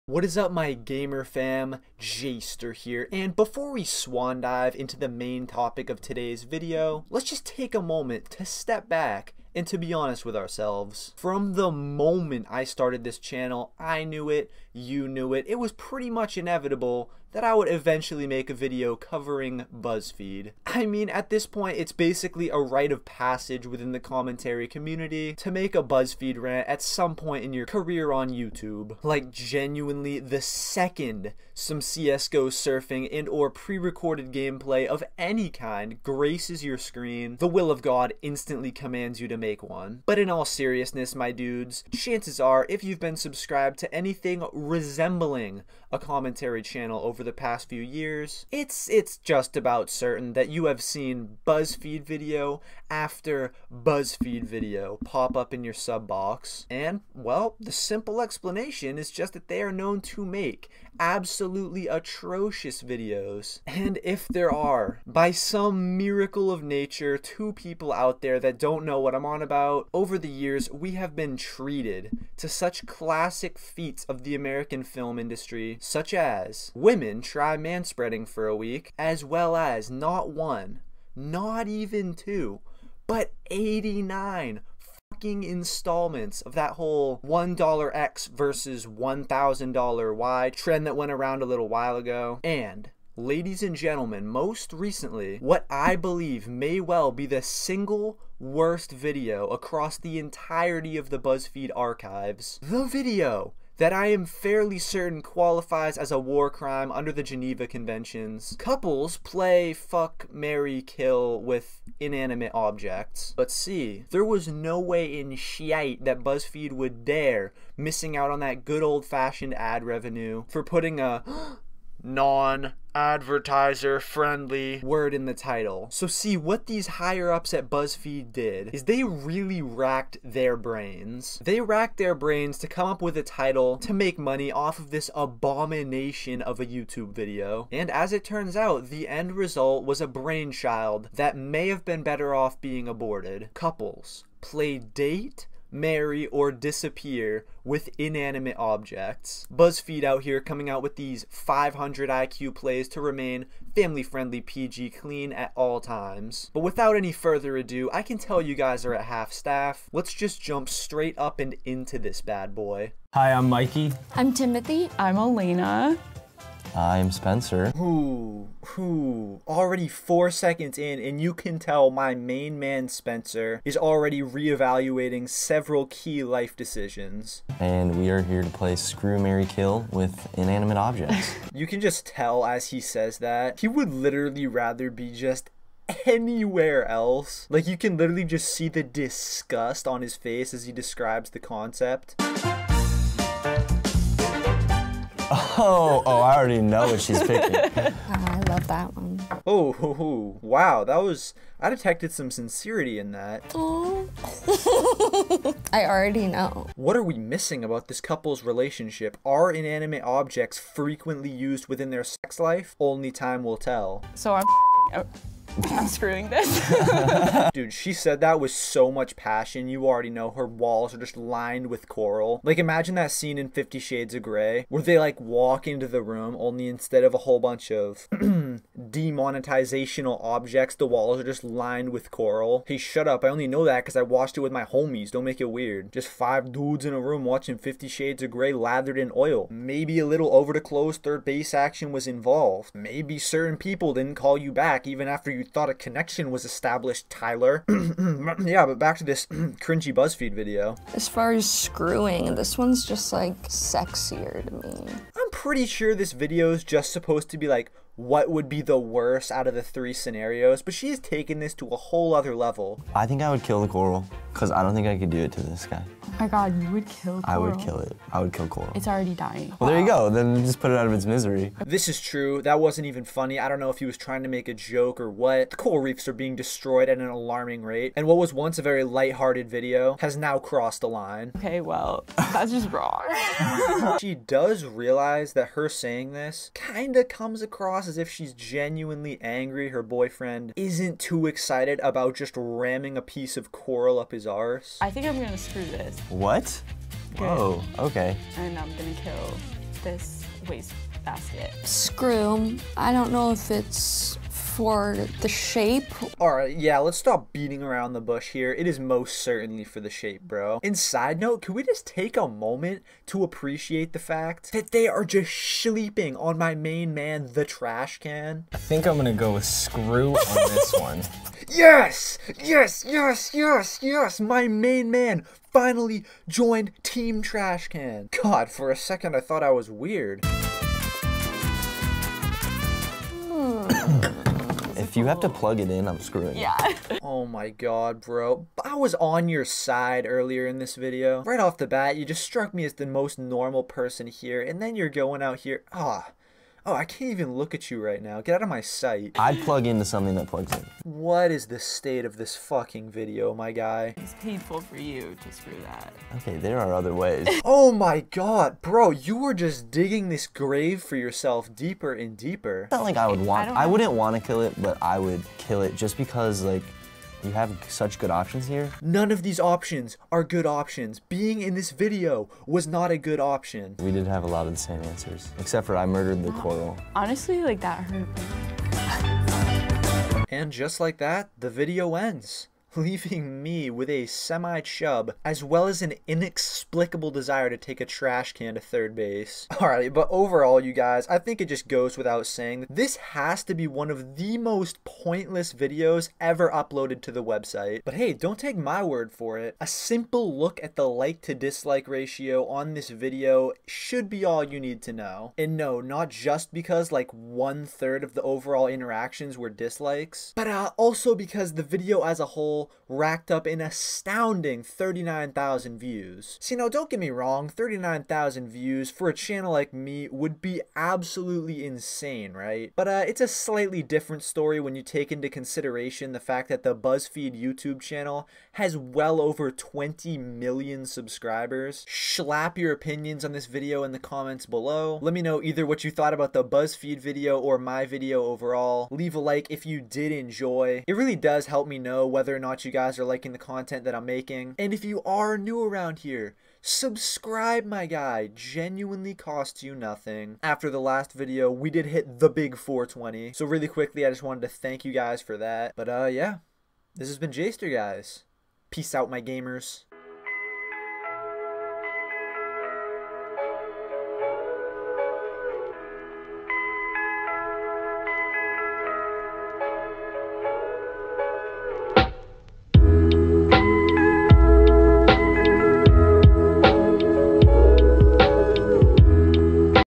The Worst Buzzfeed Video Ever (WHY DOES THIS EXIST?)
Like all great commentary channels before me, I decided to swallow my pride and take a dive into the cesspool that is the BuzzFeedVideo YouTube channel... and found the single worst video on the entire website. Peace :)